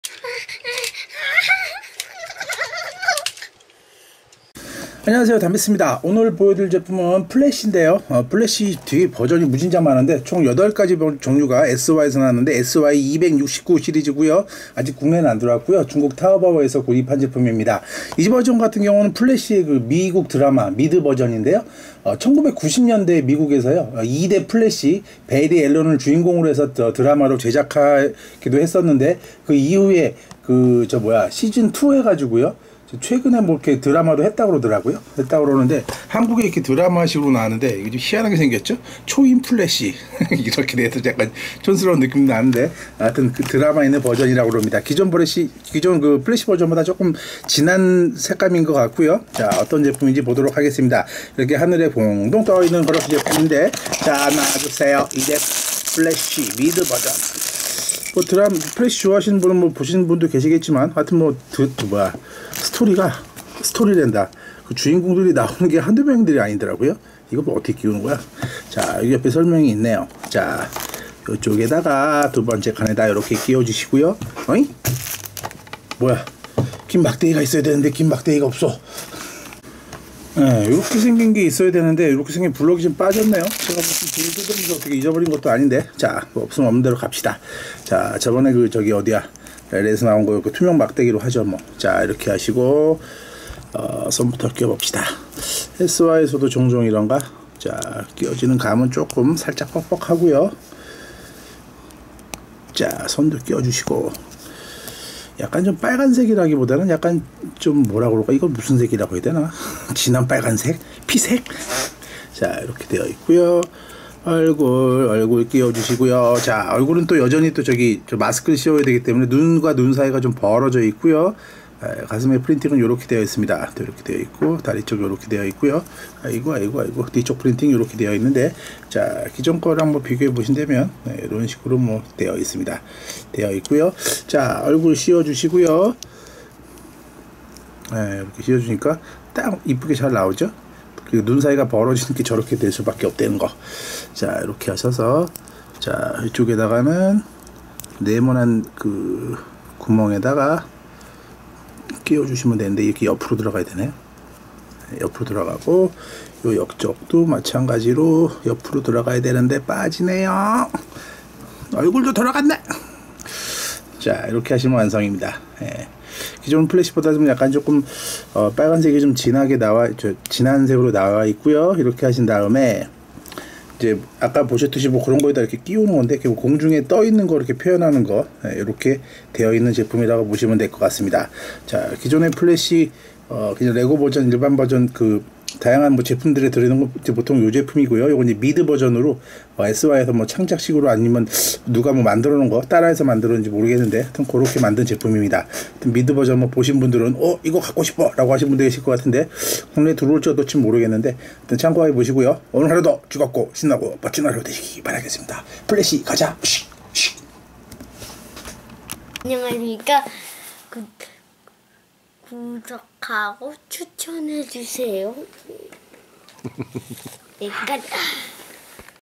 sy 플래시 미국드라마 미드버전 배리 앨런 미니피규어 레고 짝퉁 리뷰 Lego knockoff The Flash mini figures
플래시는 실험실에서 터진 알 수 없는 사고로 인해 법의학자 배리 앨런이 번개와 화학물질을 뒤집어쓰면서 우연히 스피드 포스의 강력한 힘을 갖게 되었어요. 번개같은 속도로 건물 사이를 뛰어넘고 바다를 건너고 전 세계를 누비는 그는 이제 플래시라는 이름으로 더 유명하답니다. 그는 세상에서 가장 빠른 인간으로서 끊임없이 동분서주하며 오직 범죄자를 번개처럼 빠르게 붙잡아 가둔다는 하나의 목표만을 바라보고 있다는 캐릭터로 중국 레고 짝퉁 제조 업체인 sy에서 8종의 제품 sy269를 내놓았습니다.

레고 버전, 미국 드라마 미드 버전, 리버스 플래시 빌런, 리부트 이후 플래시, 미래 버전 등 검정, 노랑, 빨강,파랑,흰색 등 다양한 색상이 출시가 되어 있네요.

1990년에 미국에서 2대 플래시 배리 앨런을 주인공으로 하여 드라마로 제작되어 방영 되었는데 한국에서는 '초인 플래쉬맨'으로 비디오로 출시 되었고, 이후 MBC에서《형사 플래시》라는 제목으로 방송하며 나름대로 인기를 끌기도 했다.

DC 코믹스의 히어로인 2대 플래시 배리 앨런을 다룬 드라마. 주연은 그랜트 거스틴. 같은 DC 코믹스 원작 드라마인 《애로우》의 스핀오프로서 시작되었으며, 이후로도 세계관이 연동되고 있다.

플래시는 DC 코믹스에서 상당히 비중이 있는 주요 히어로인데도, 1990년 존 웨슬리 십 주연으로 제작된 드라마(한국에는 '초인 후뢰시맨플래쉬맨'이라는 제목으로 방송되었다.) 외에는 실사화에 관한 별다른 소식이 없었다. 그러던 중 뜬금없이 《애로우》의 시즌 2 8~9화에서 배리 앨런이 등장하며 플래시의 실사화 가능성이 제기되었고, 곧 공식 발표가 뜨면서 현실이 되었다

 배리는 스타 연구소에서 진행한 입자 가속기 실험의 사고로 인해 발생한 번개에 맞고 9개월간 혼수 상태에 빠지게 된다. 폐쇄된 스타 연구소의 연구진들은 깨어난 배리에게 배리가 초인적인 스피드와 힘을 얻게 되었다고 말하고, 이후 배리 외에도 입자 가속기 실험 사고로 인해 초인적인 능력을 얻은 인물들이 있다는 것이 밝혀진다. 배리는 이전에 만났던 애로우의 조언을 듣고플래시가 되어 센트럴 시티를 지키기로 결심한다.